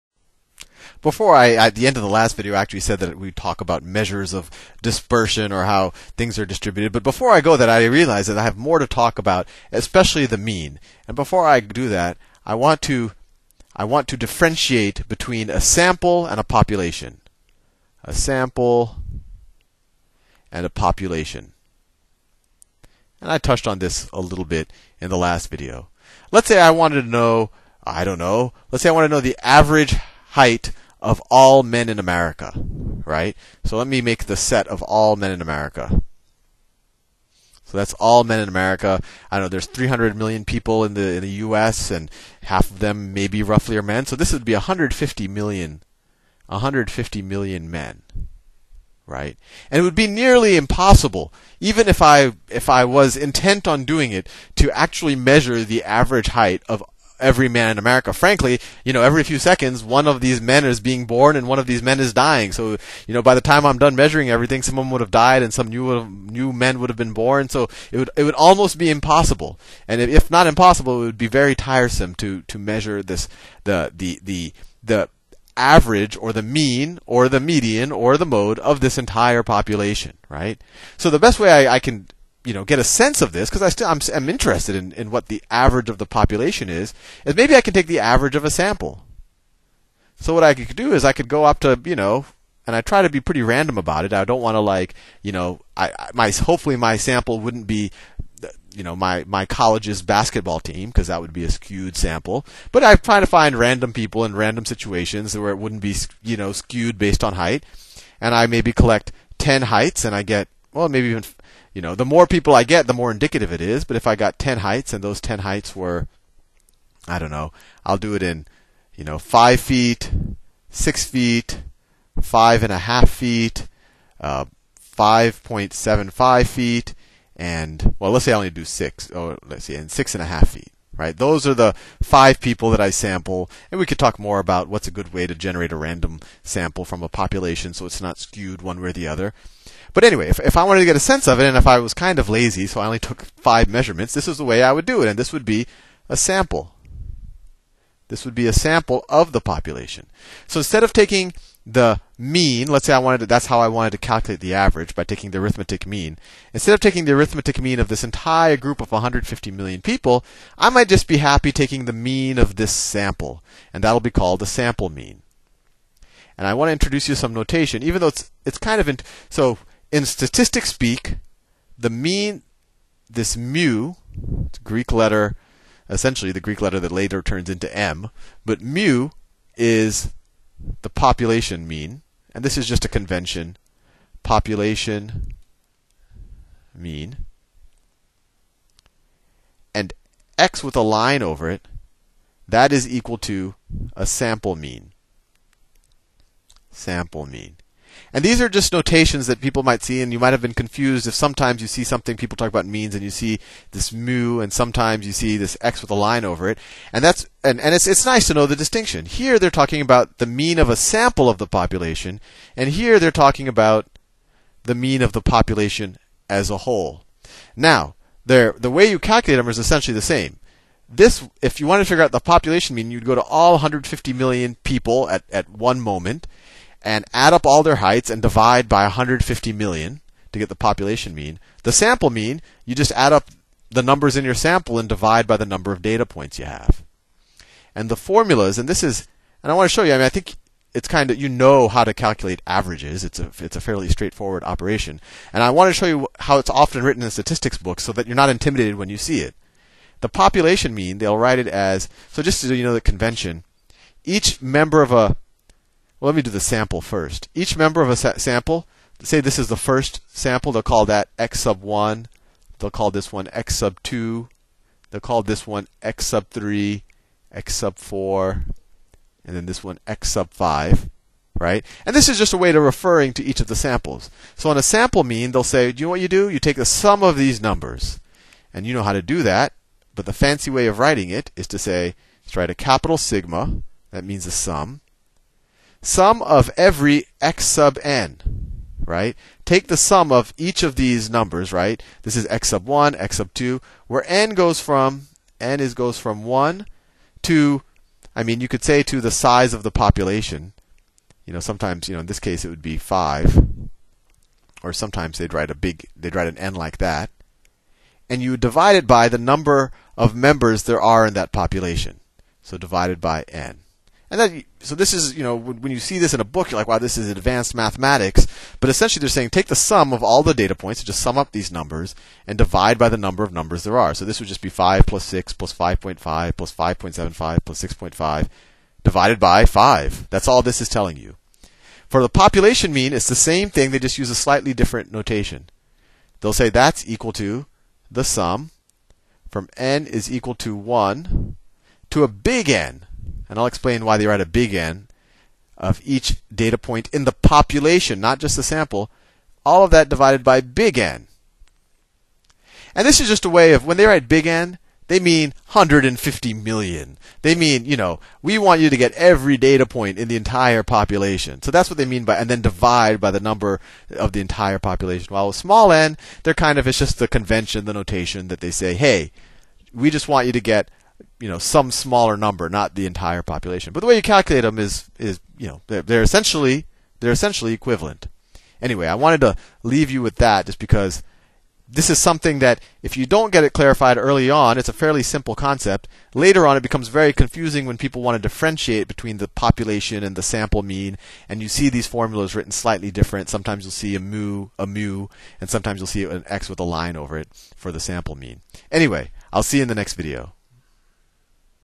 Statistics: Sample vs. Population Mean
The difference between the mean of a sample and the mean of a population.